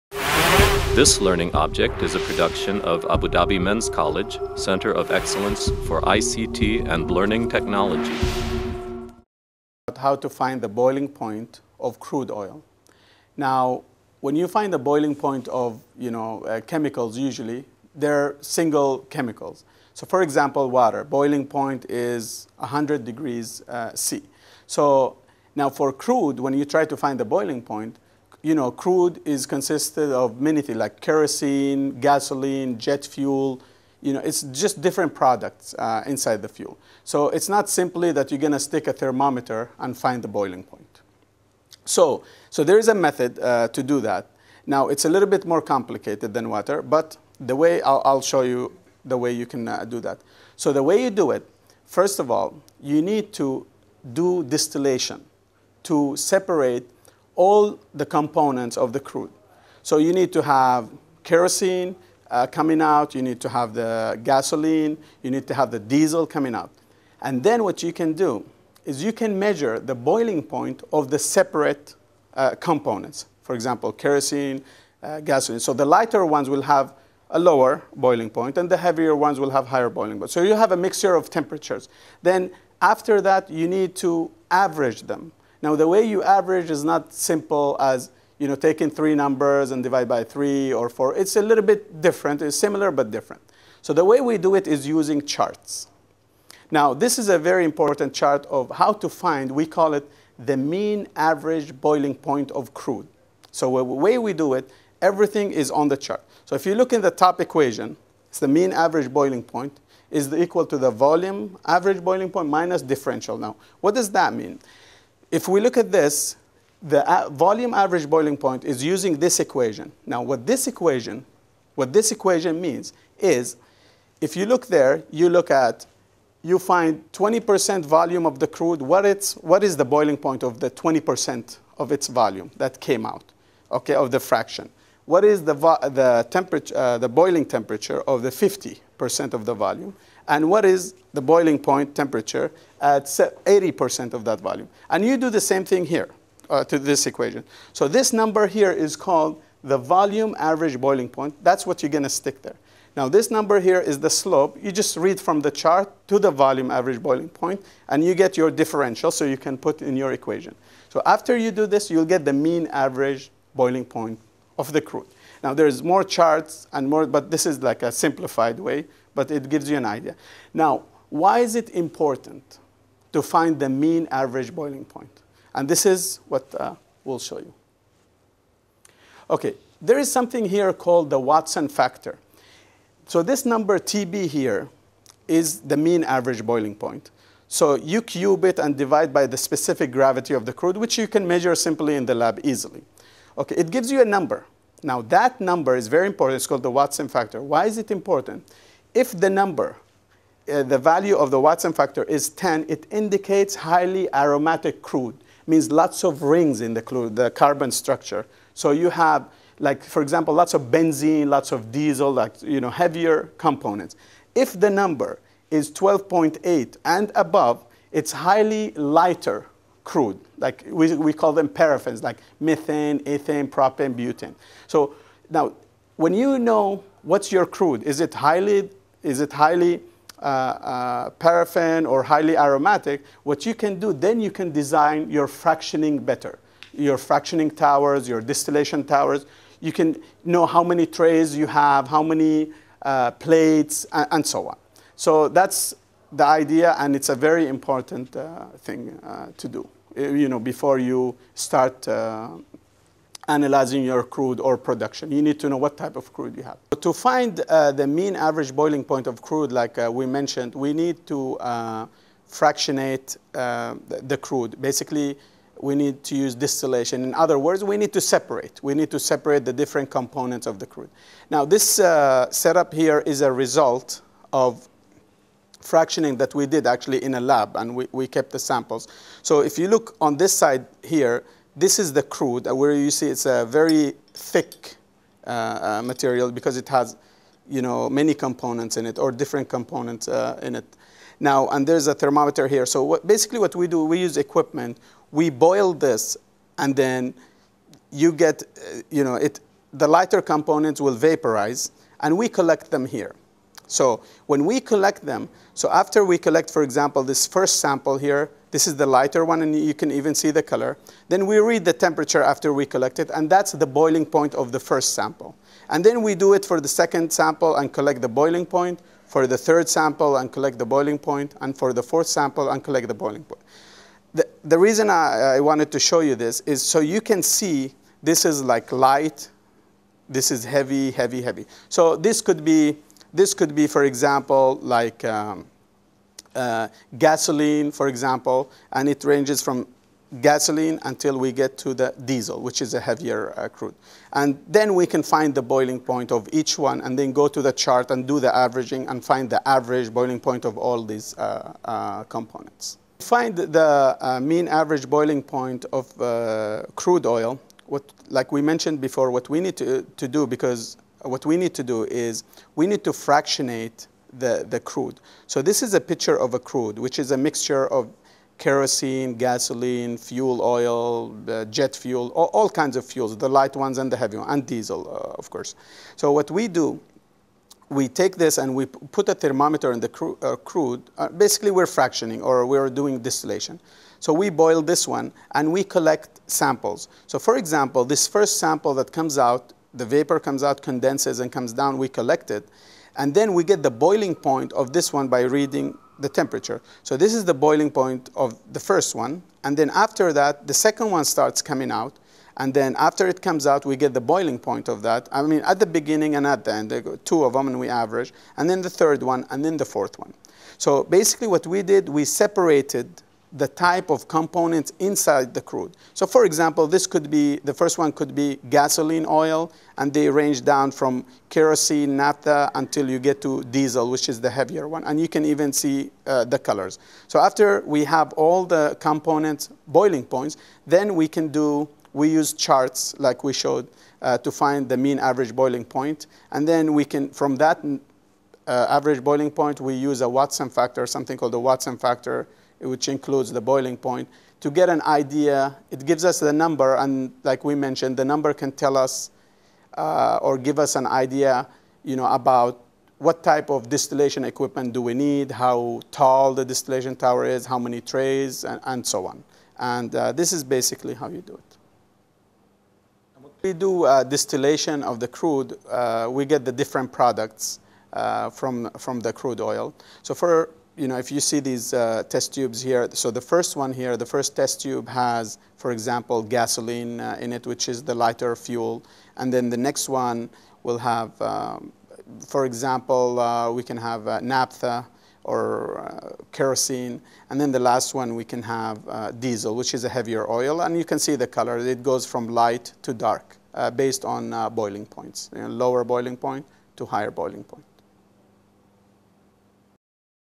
Crude Oil Distillation
This video looks at the basics of distilling crude oil into a variety of fuels.